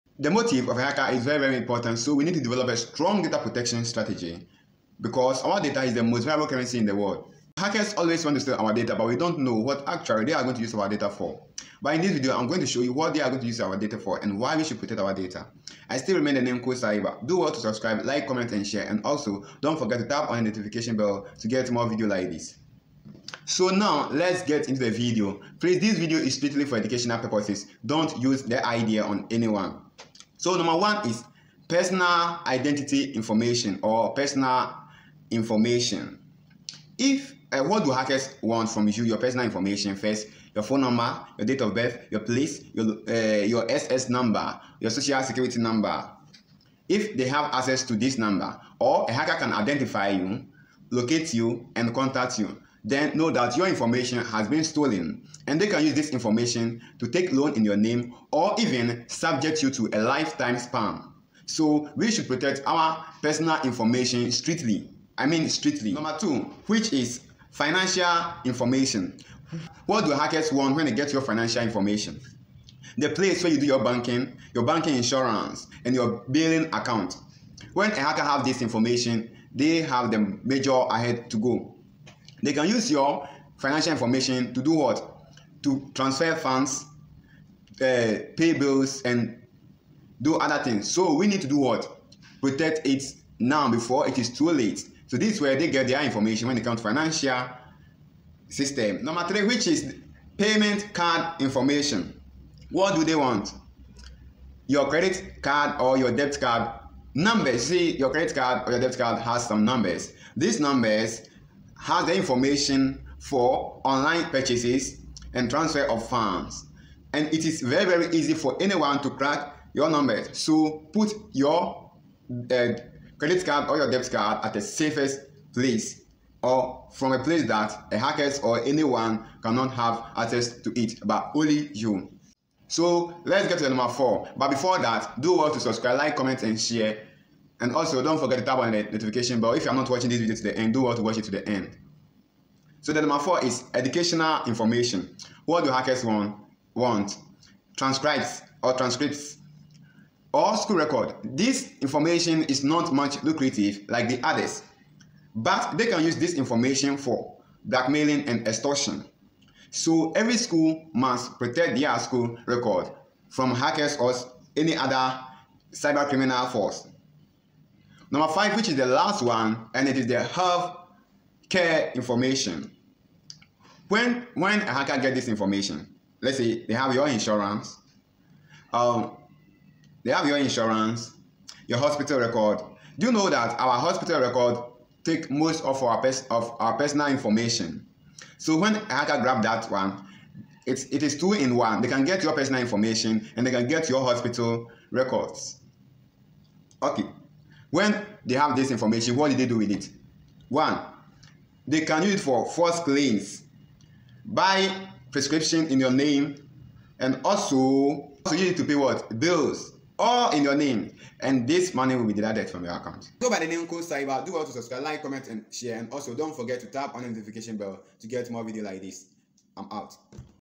What hackers want and why they hacked
in this video I have listed the various categories that shows you where hackers get their information so do will to watch the video to the end. don't forget to subscribe @koscyber  like comment and share the video with your friends so that everyone will be alert.

Follow koscyber on social Media platform as Koscyber